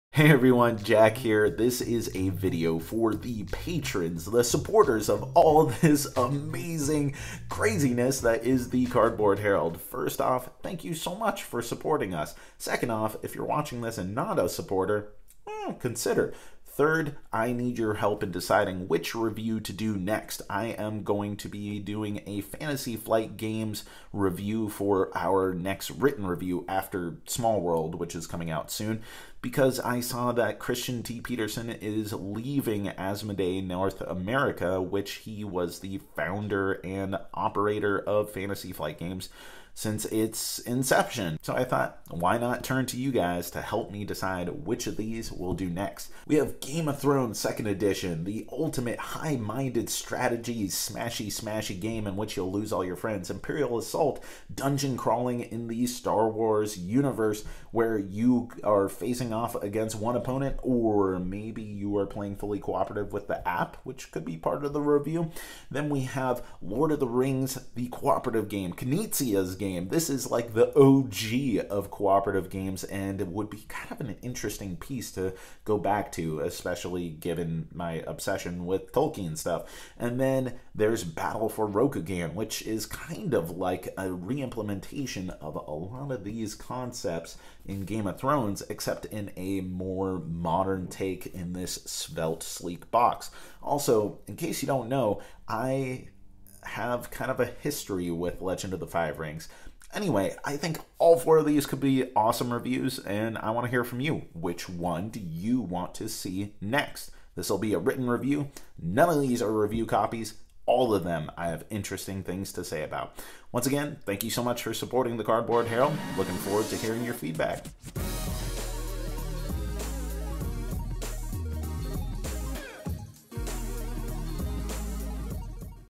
Patreon Vote - Next game to review
In light of FFG founder Christian T Petersen's announcement that he is stepping down as CEO of Asmodee NA, I thought I'd throw it to the Patreon backers to help me decide what game to review in writing next.